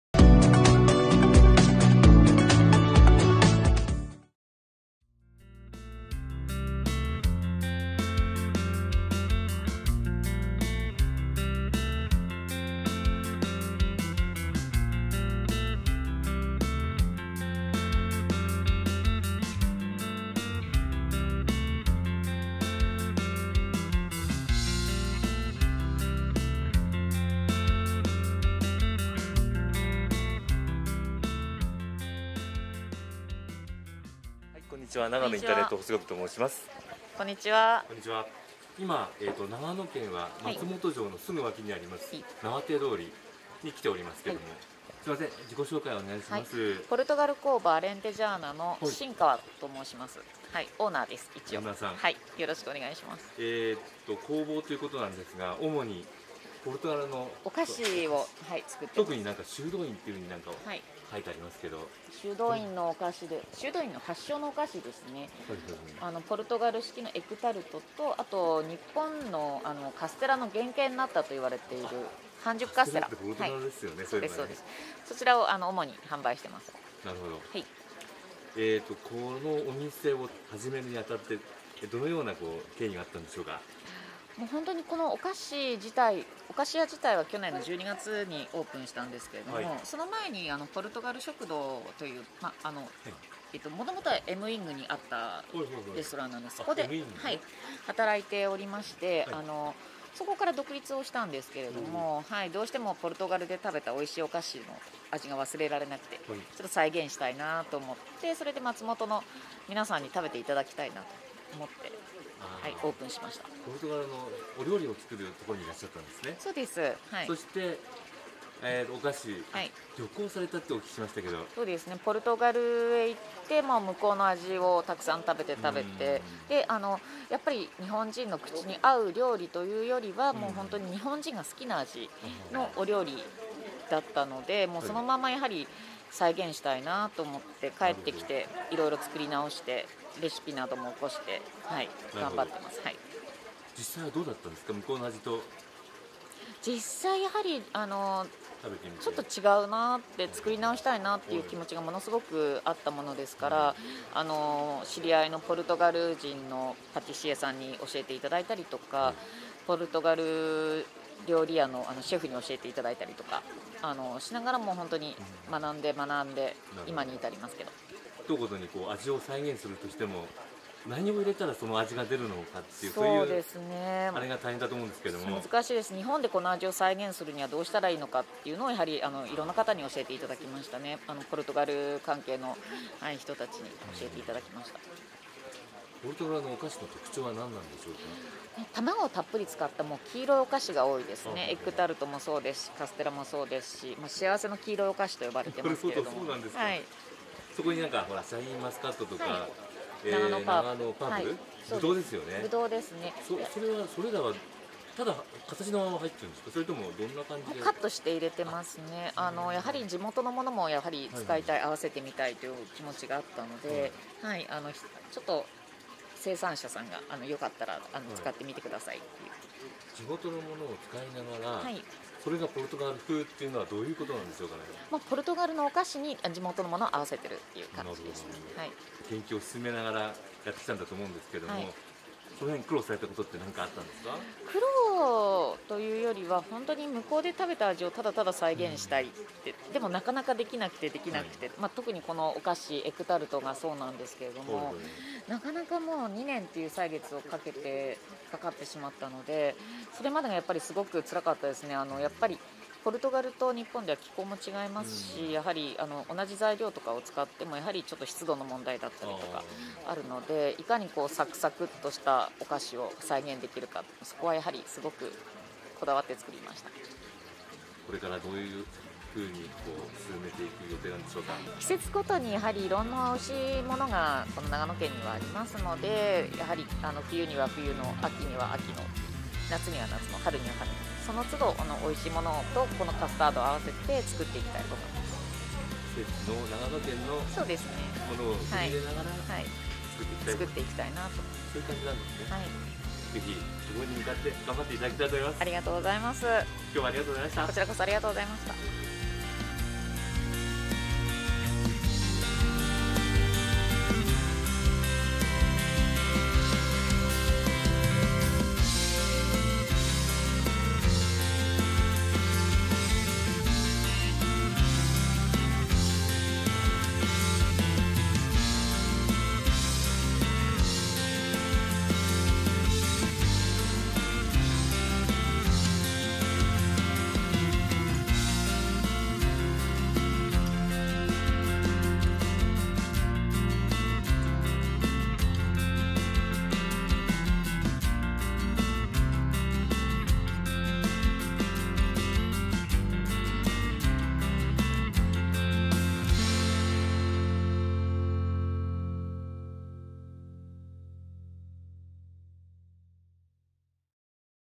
ポルトガル工房: アレンテジャーナ:ポルトガル修道院伝統菓子を忠実に再現！
ポルトガル工房

ポルトガル伝統菓子を忠実に再現！サクサク生地に濃厚クリームが絶妙
松本市内のはしご横丁からなわて通りに移転オープン。ポルトガルの伝統焼き菓子で「ナタ（パステル・デ・ナタ）」と呼ばれるエッグタルトを販売。タルト部分はパイ生地で、中にカスタードクリームが入る。パリパリの生地にとろりんクリームのコンビネーションが絶妙。食べ歩きにちょうどいいサイズで手土産にもおすすめ！！

by デリシャスコマチより